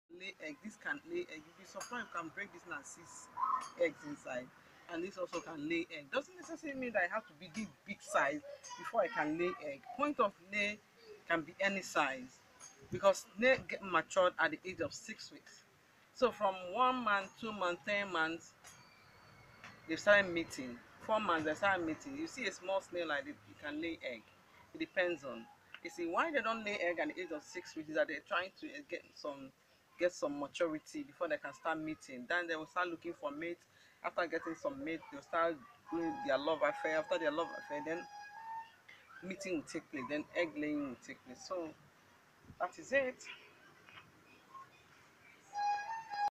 How many months does it take for a snail to mature. point of lay. elevage d'escargot
The early maturity of a snail is essential in snail farming as it allows you to start making profit out of snail farming.

Snails are farm animals which take a lot of time to grow since they are sometimes not fed with the right amount of nutrients.

This video is one where I visited a snail farmer who has farmed snails for quite a while and has gained experience from the farm.

She explains to me how long it takes for a snail to.mature and the meaning of point of lay snails.

This video shows the mature age of a snail.